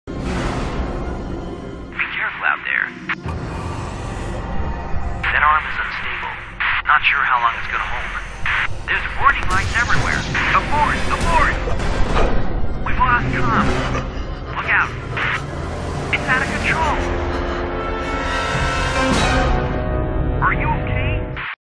Gravity (2015-05-09) Short Movie
Logan Xavier stars in this short remake of the award winning blockbuster!